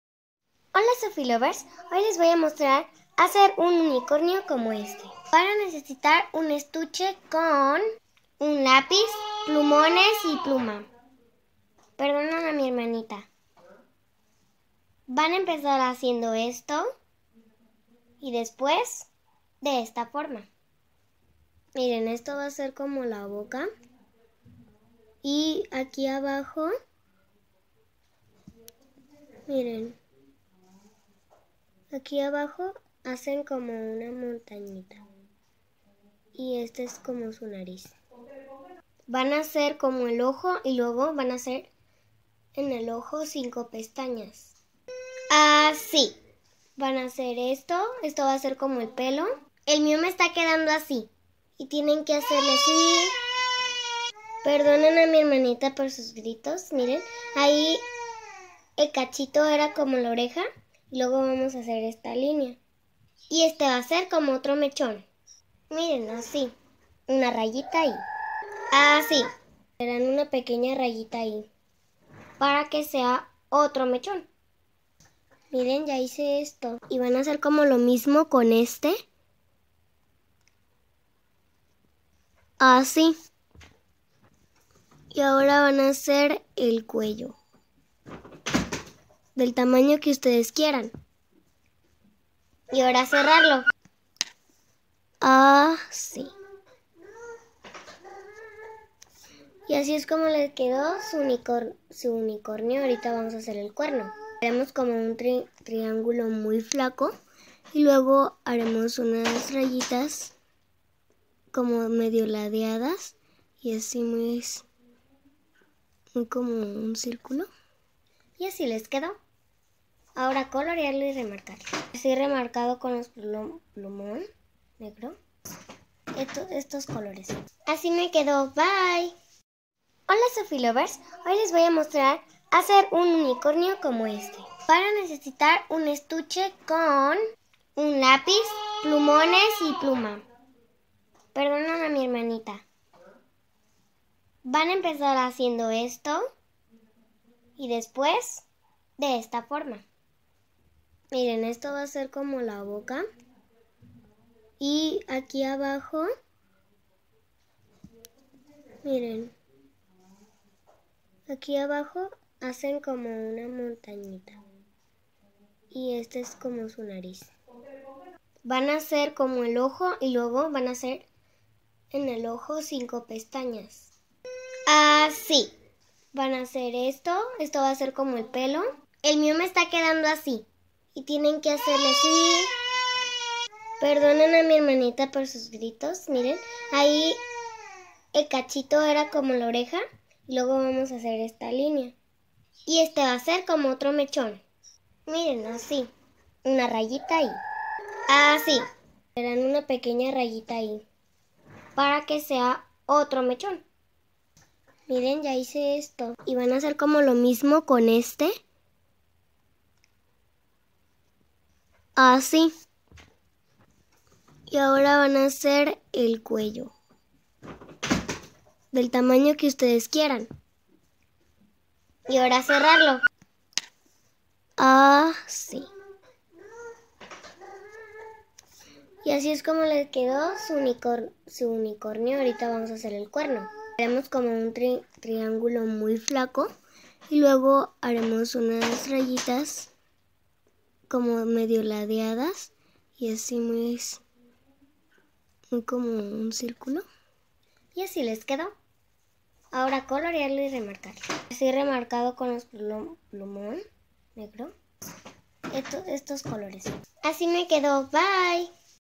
Como se dibuja un unicornio/ de shopie medina
Hola SOFILOVER S como están espero te sirva este dibujo para impresionar a tu familia de que eres un artista del arte 😂😂 ¡mucha suerte !